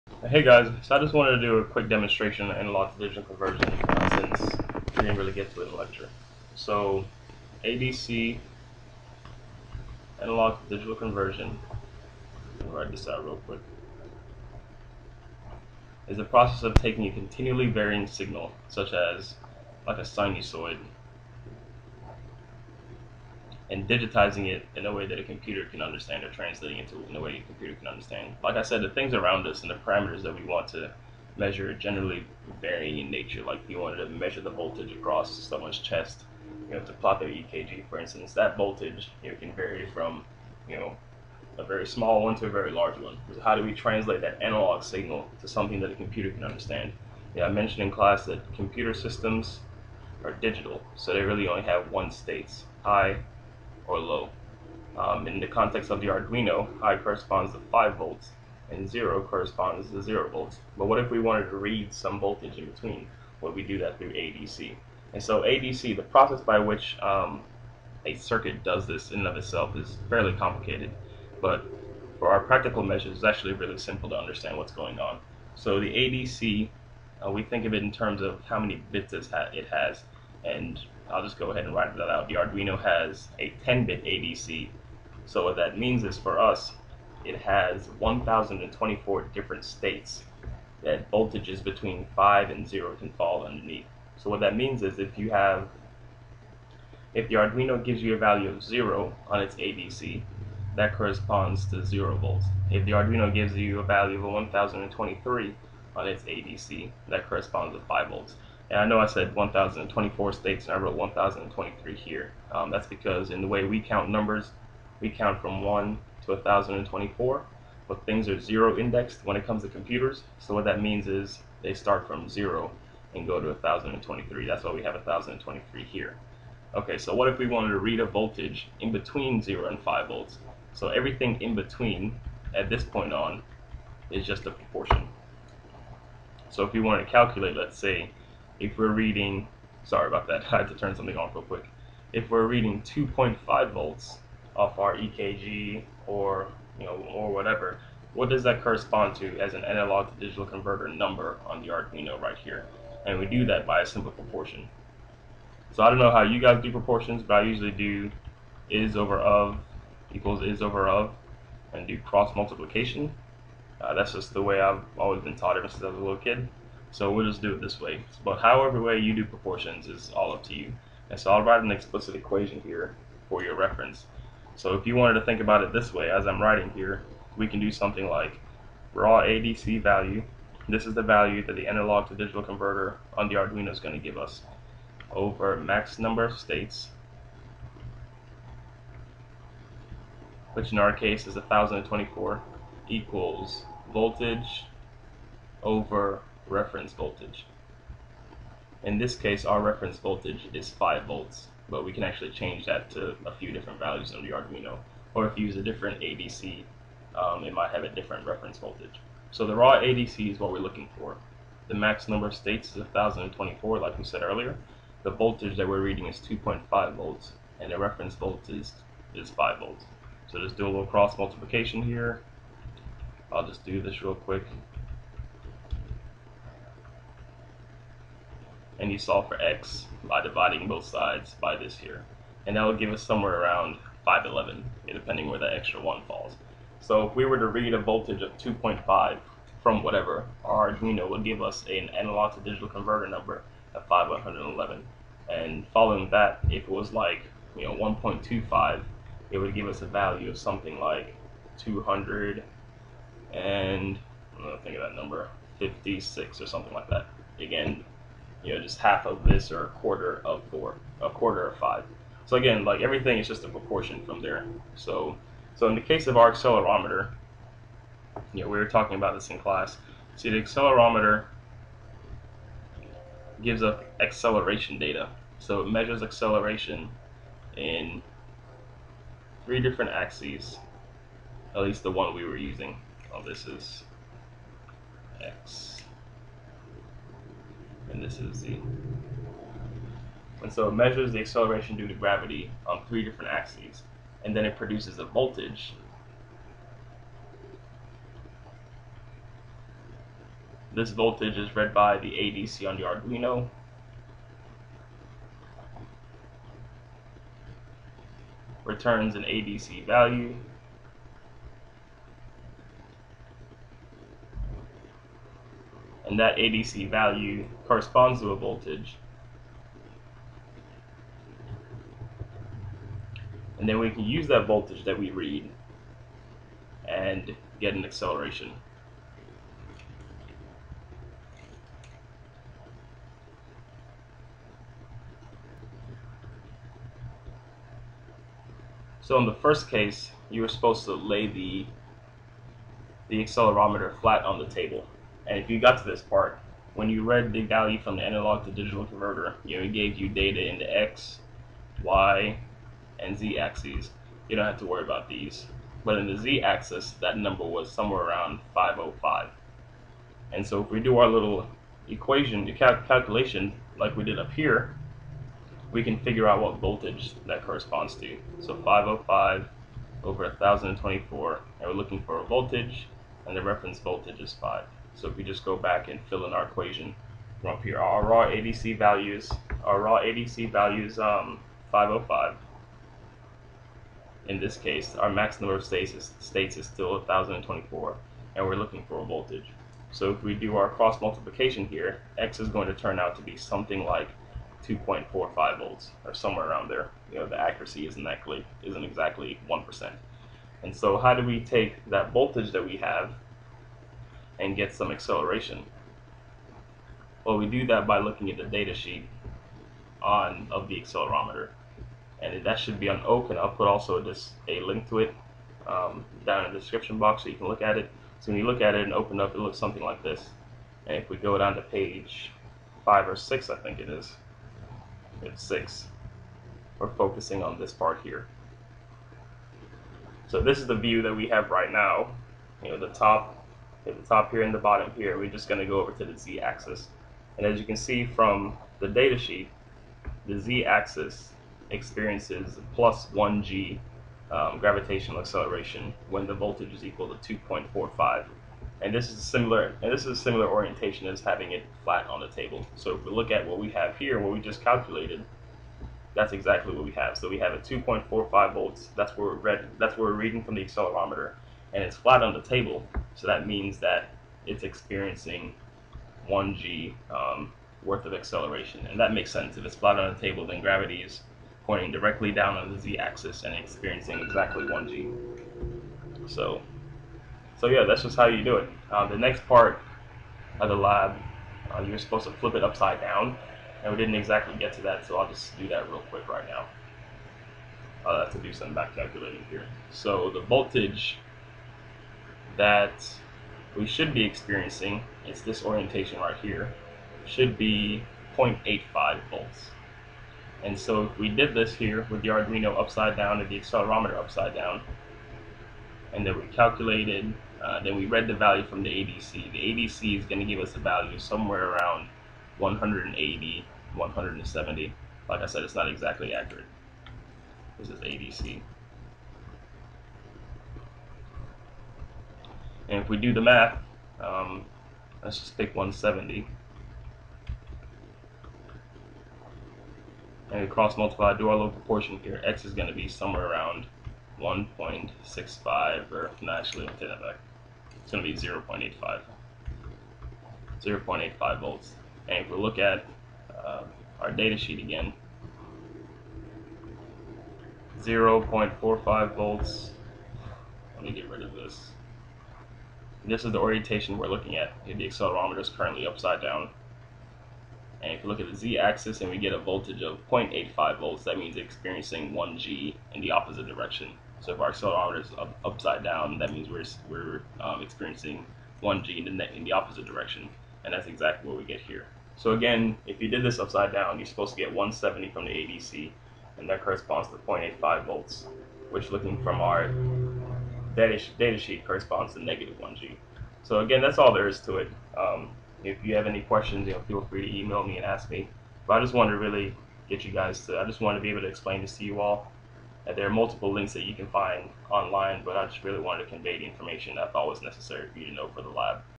Analog-to-Digital Conversion Tutorial and Accelerometers
I created this video as an introduction to analog-to-digital conversion for biomedical engineering students I am teaching in an intro to engineering class at Vanderbilt University. The tutorial was created in light of a lab activity the students had to complete using an accelerometer. Very simple explanation. I hope that by making this video public, others can benefit from the explanation as well.

You may download the PDF version of the notes that I am writing